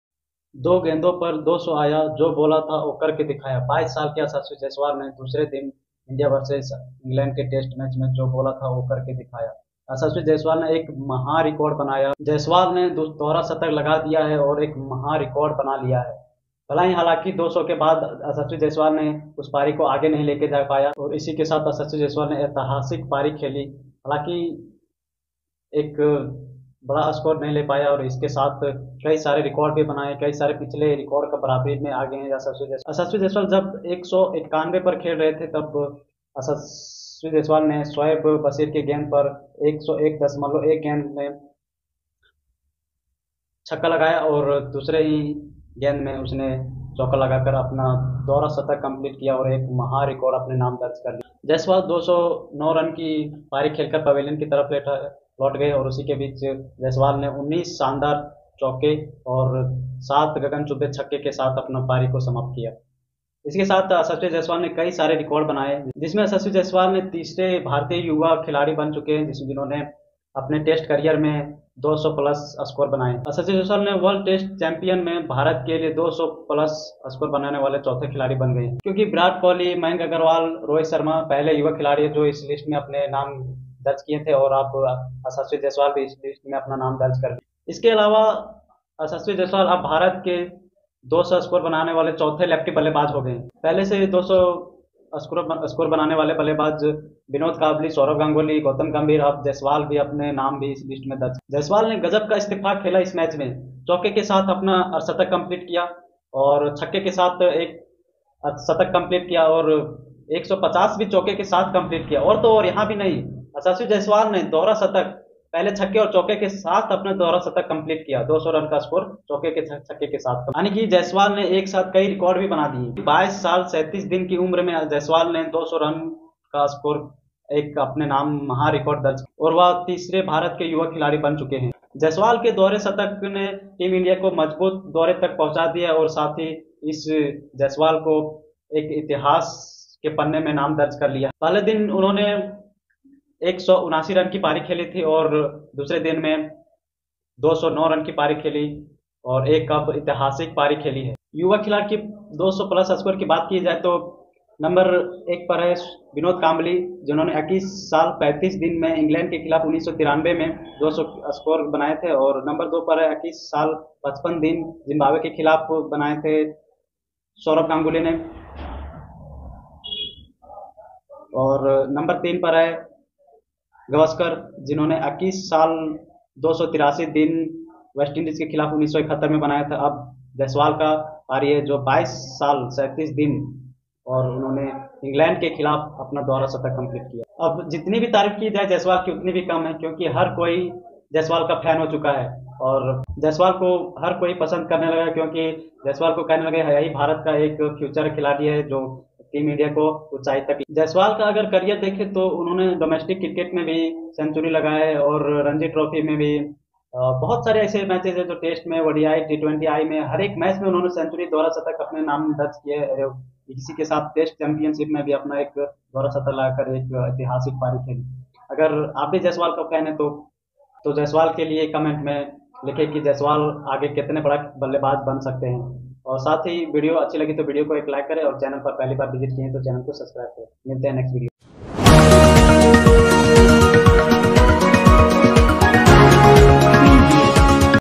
INDVSENG 2ND TEST YASHASVI JAISWAL ने DOUBLE CENTURY BANAYA और एक महा रिकॉर्ड
Jaiswal Ne Kamal Ke ballebaji ki
Jaswal Ne dohra shatak Lagaya aur Kai Sare new record
Ek Shandar team India Tak ka score Lekar Gaya aur aur team 396 ke score per pahunchaya